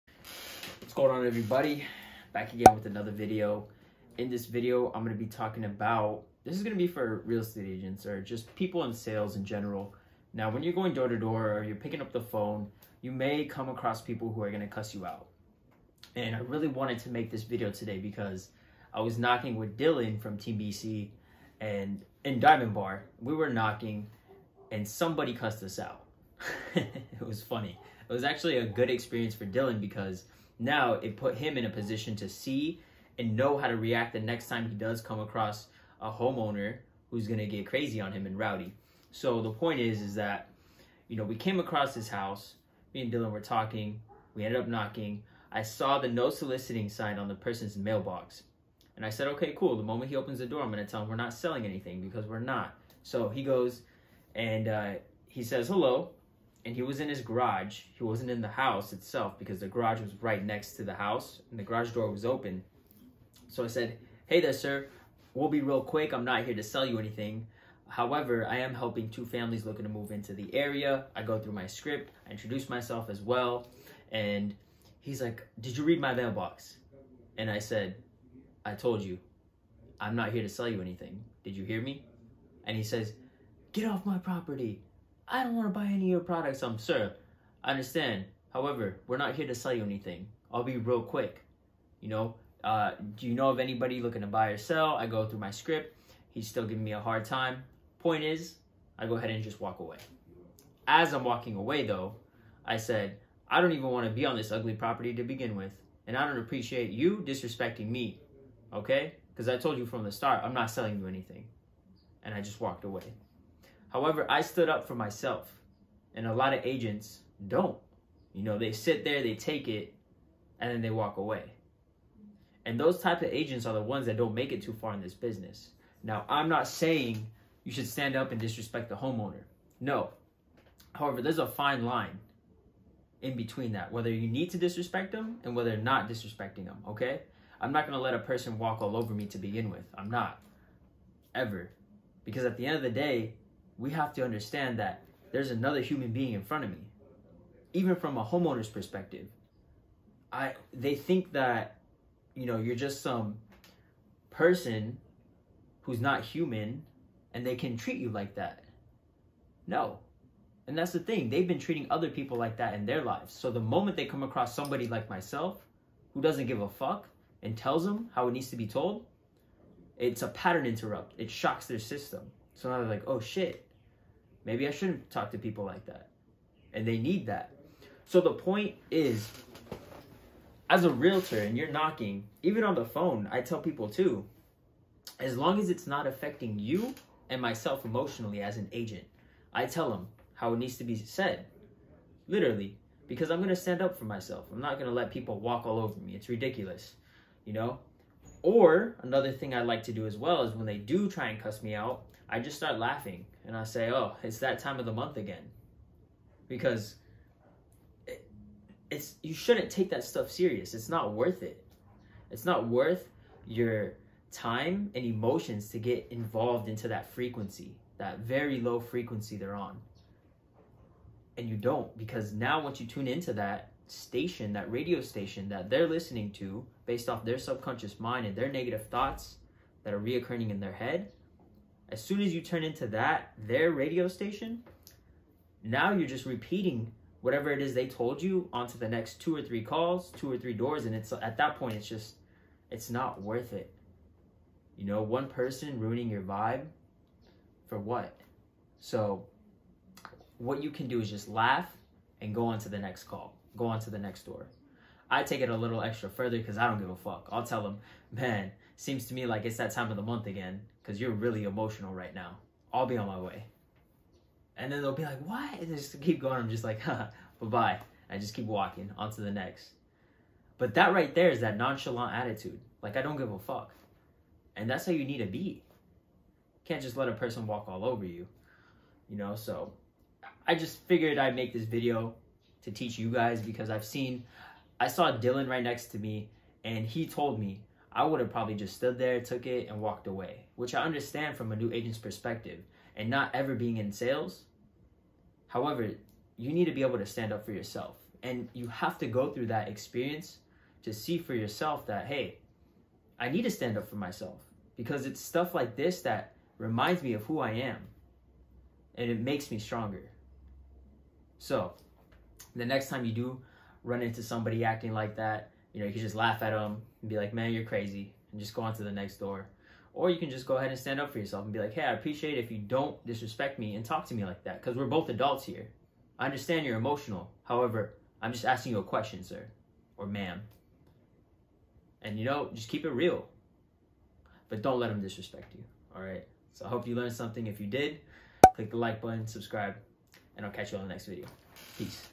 Getting Cussed OUT while door knocking
In this video i talk about how I recently got cussed out while door knocking, and I explain how to handle angry prospects.